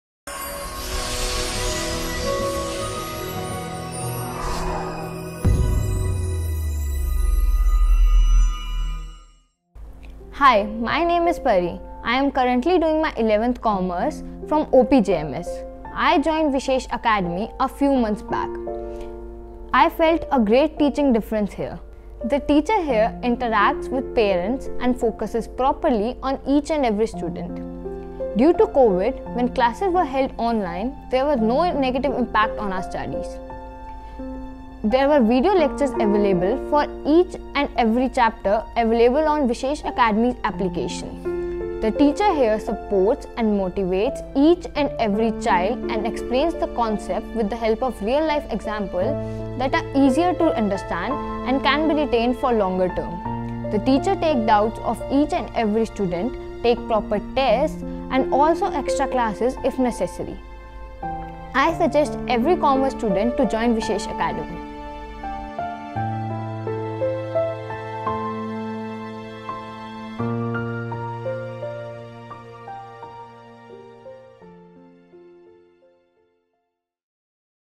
Student Feedback | For Commerce only One Solution {vishesh academy} | Join us
Testimonial of Pari - A student of Vishesh Academy of Commerce

Follow us for more @visheshacademyofcommerce